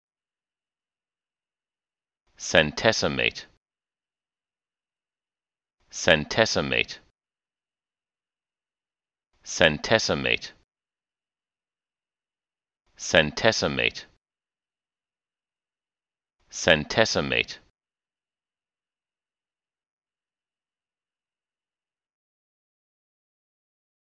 How to pronounce centesimate
centesimate American English pronunciation. How to pronounce centesimate correctly. How to say centesimate in proper American English.